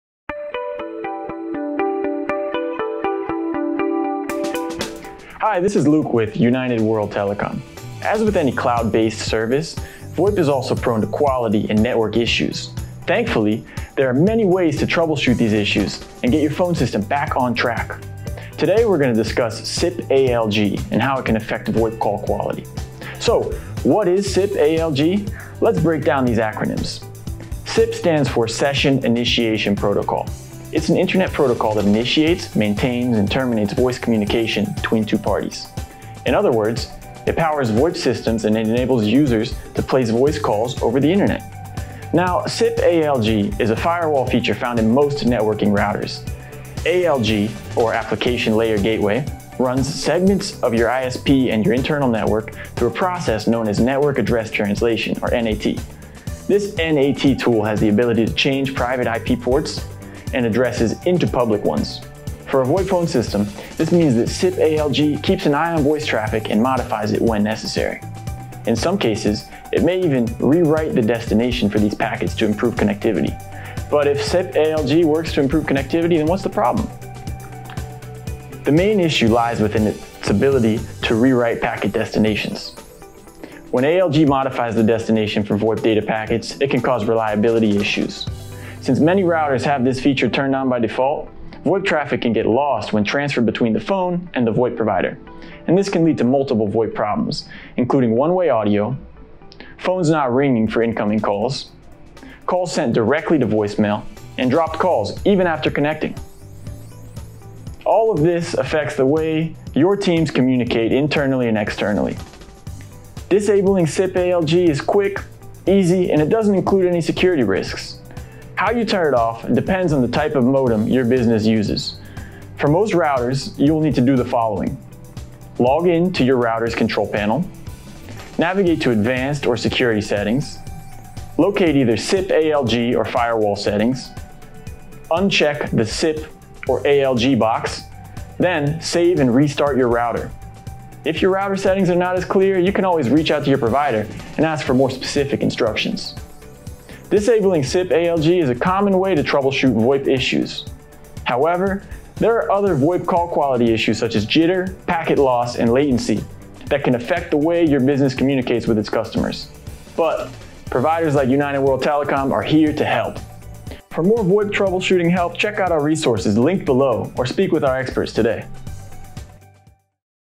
What is SIP ALG and Why Should You Disable it?
As with any cloud-based service, VoIP and SIP phone systems are also prone to quality and network issues.

Thankfully, there are many ways to troubleshoot these issues and get your phone system back on track.

This video explains what SIP ALG is and how it can affect VoIP call quality.

SIP ALG is a firewall feature found in most networking routers.

Session Initiation Protocol (SIP) is an internet protocol that initiates, maintains, and terminates voice communication between two parties. It powers VoIP systems and enables users to place voice calls over the internet.

Application Layer Gateway (ALG) routes segments of your ISP and your internal network through a process known as Network Address Translation (NAT). Through these processes, SIP ALG  watches your VoIP traffic and modifies it when necessary. It may sometimes even rewrite the destination for these packets to improve connectivity.

This ability to rewrite packets can cause call reliability issues. And since many routers have this feature turned on by default – VoIP traffic can get lost when transferred between the phone and the VoIP provider.

Watch the video to learn how to disable SIP ALG.

For more VoIP troubleshooting help or advice, visit United World Telecom.com and chat with our telecom experts today at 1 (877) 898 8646!

TIMESTAMPS
Intro 00:00
What is SIP ALG? 00:25
Should VoIP Users Disable SIP ALG? 01:15
How to Disable SIP ALG 01:52